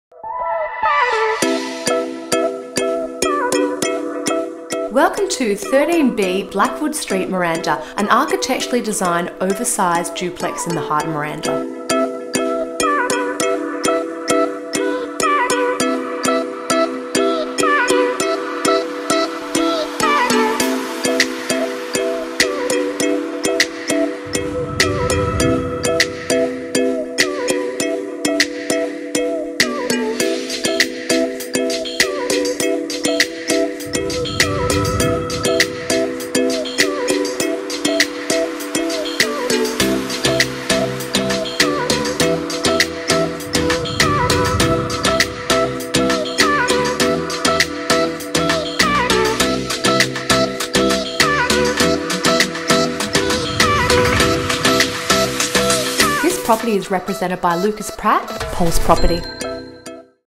Miranda | 13B Blackwood Street | Pulse Property Agents
Peacefully set in a quality street within walking distance to Westfield and showcasing luxurious inclusions, this brand new oversized Torrens Title duplex offers executive-style living with elegant indoor/outdoor entertaining and is bathed in natural light courtesy of its North-facing rear.